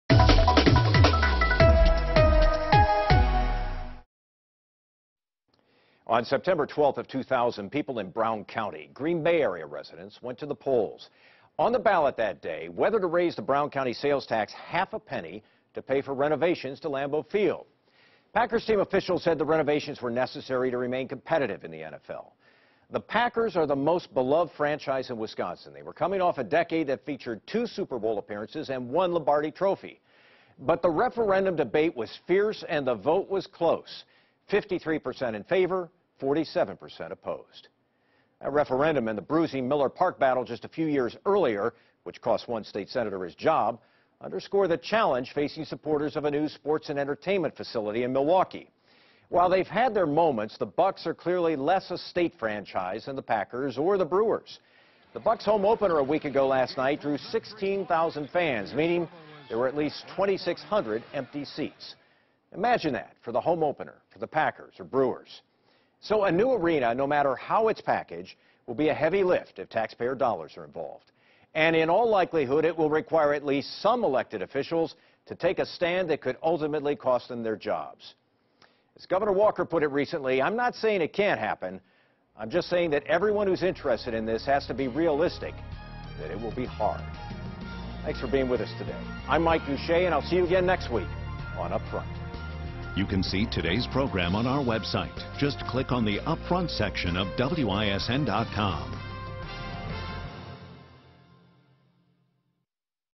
Packers, Brewers fought for stadium remodeling, replacement
Mike Gousha says the experience of other sports teams in seeking public funding for stadiums suggests that backers of a new Milwaukee arena will have a heavy lift.